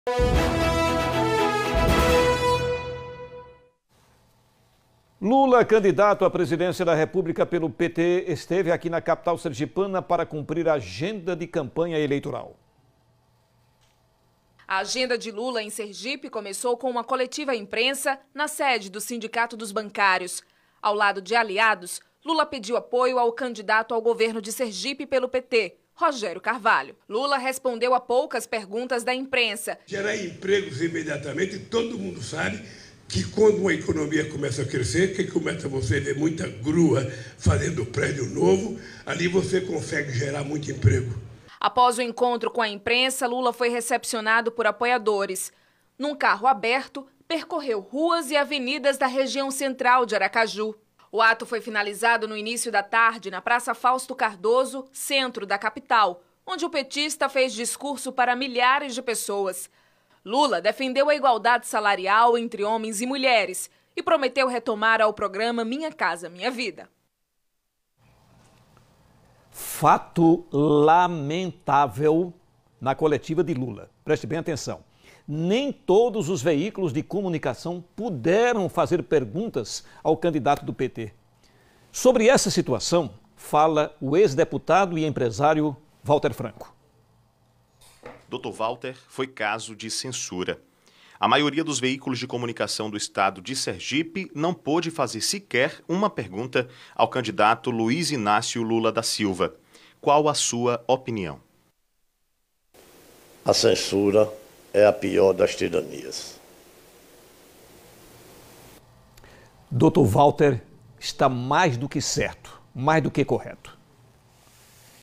Lula candidato à presidência cumpriu agenda de campanha em Sergipe - Jornal do Estado
A agenda de Lula em Sergipe começou com uma coletiva à imprensa, na sede do sindicato dos bancários de Sergipe. Ao lado de aliados, Lula pediu apoio ao candidato ao governo de Sergipe pelo PT, Rogério Carvalho.